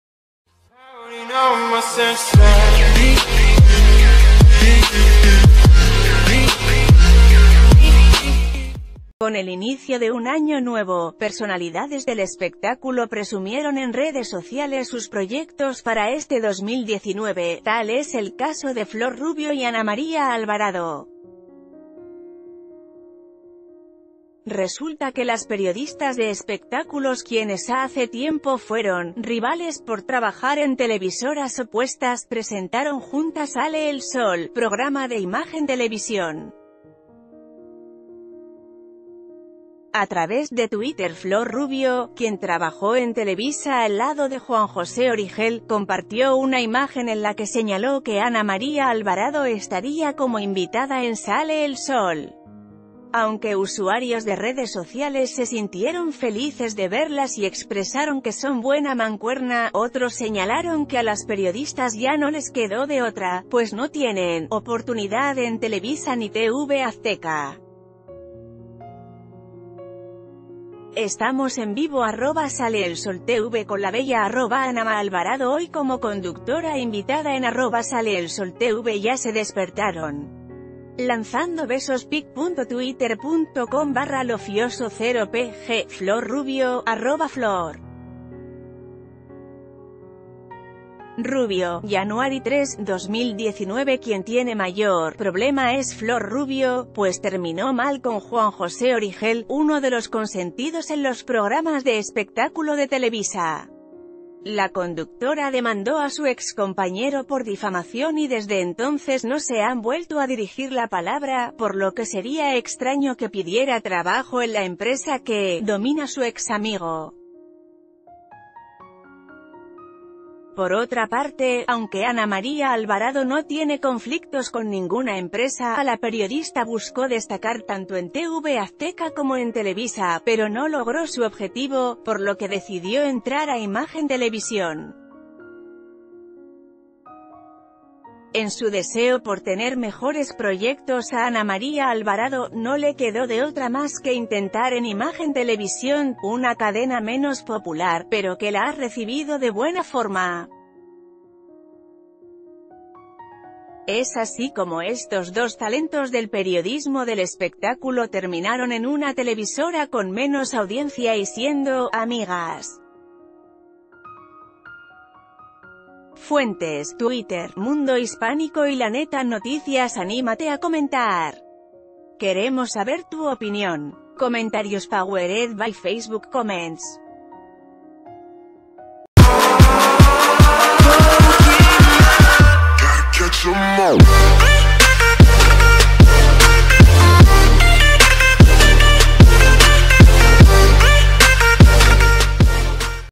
Flor Rubio y Ana María Alvado, del odio al amor en los espectáculos
Resulta que las periodistas de espectáculos, quienes hace tiempo fueron ‘rivales’ por trabajar en televisoras opuestas, present...